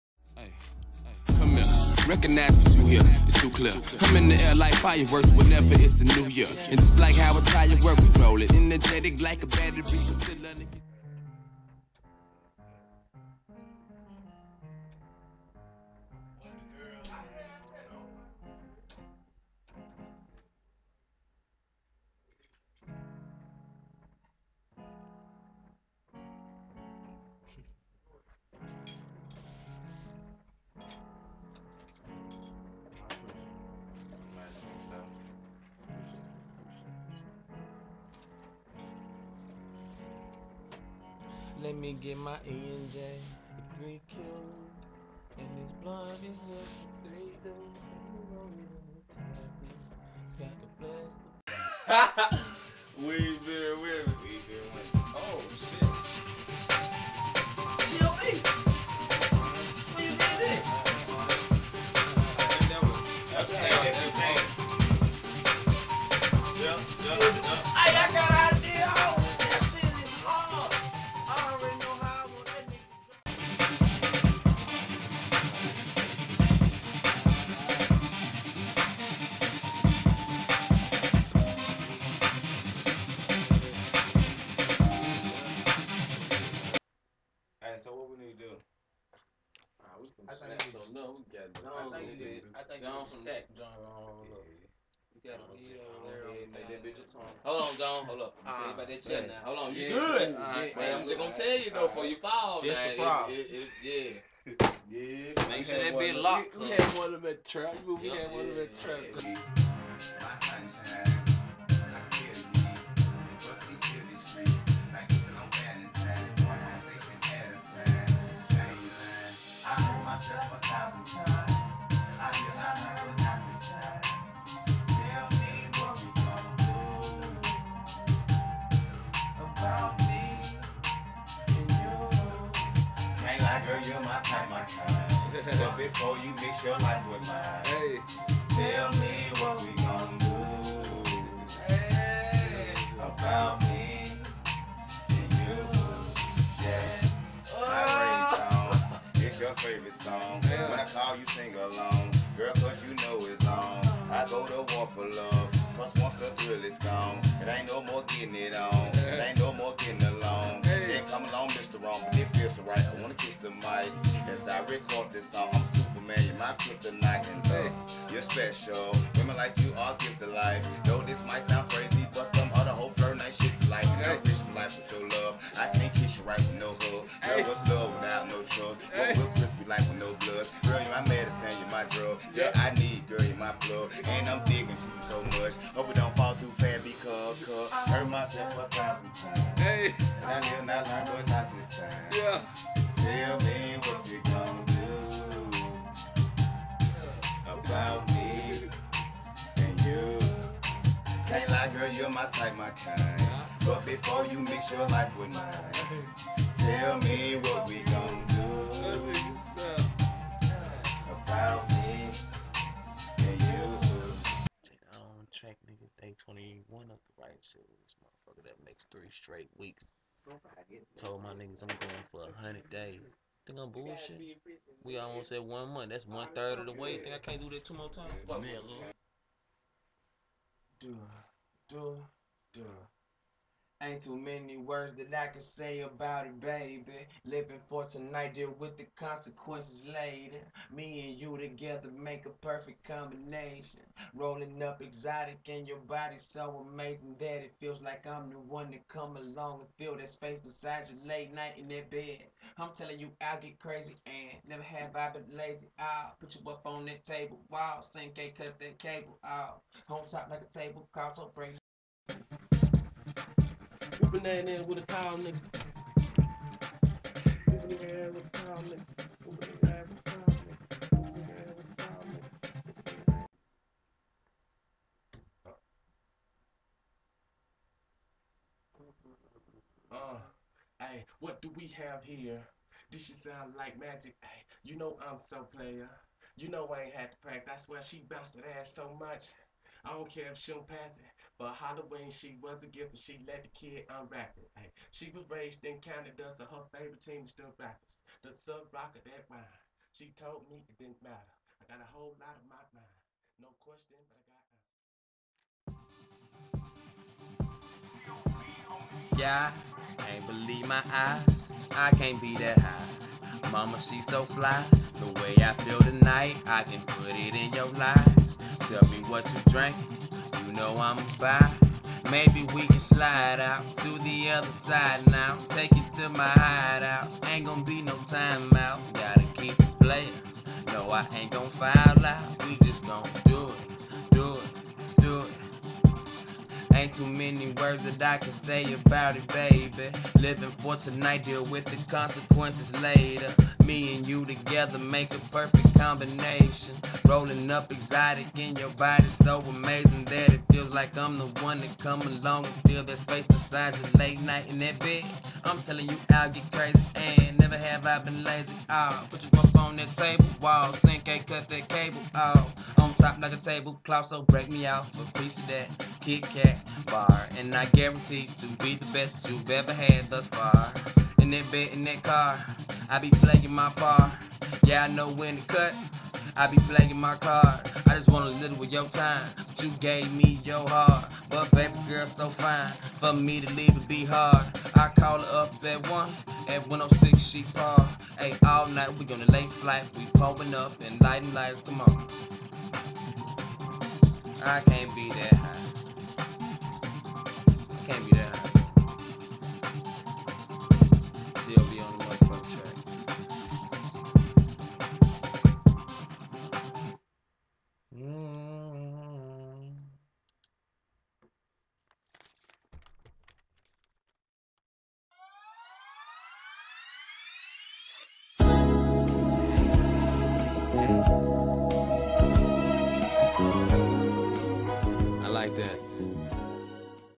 J' Still Writing Game 4: Pre-Game
2/10/13 This makes three straight weeks of daily uploads and freshly written verses/songs...the marathon continues..be on the lookout for studio recorded versions of the the songs from this writing series...SUBSCRIBE TO THE CHANNEL AND STAY TUNED